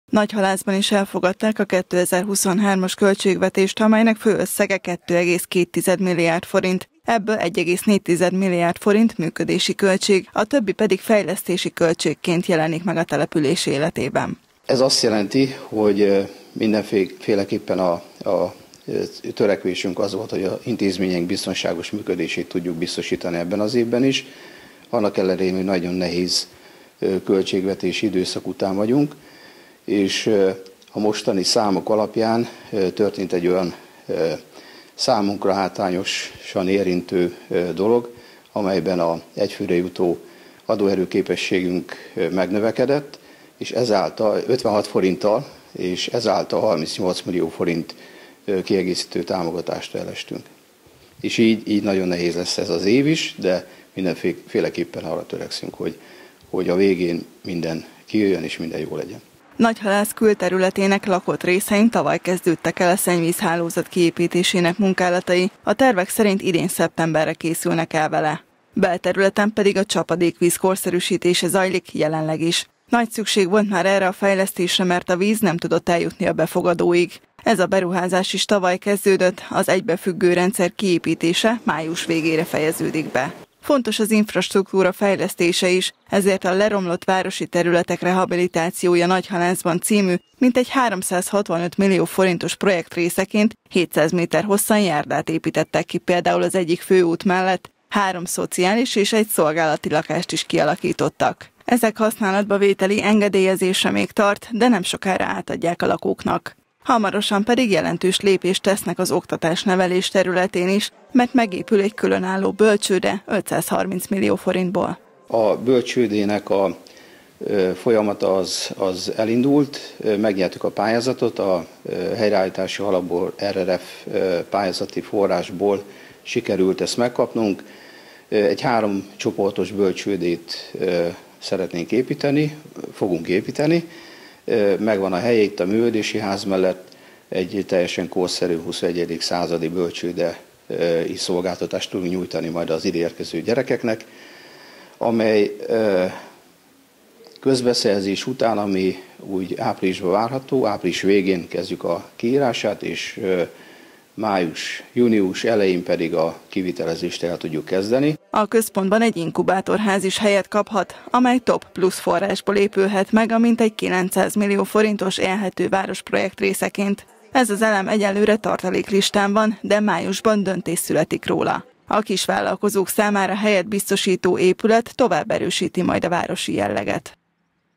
Fejlesztések Nagyhalászban
Zajlanak a szennyvízhálózat kiépítésének munkálatai Nagyhalász külterületének lakott részein. Ezzel párhuzamosan belterületen a csapadékvízhálózat korszerűsítését végzik a szakemberek. Erre több mint 300 millió forint áll az önkormányzat rendelkezésére. Hamarosan elkezdődik a bölcsőde építése is, májusban pedig arról születik döntés, hogy TOP Plusz forrásból megépülhet-e a szolgáltatói inkubátorház a település központjában